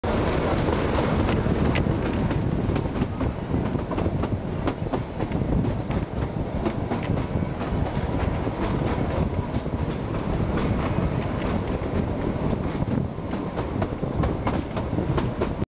Kalka to Shimla in a toy train.
wonderful journey in a toy train from kalka to shimla in toy train.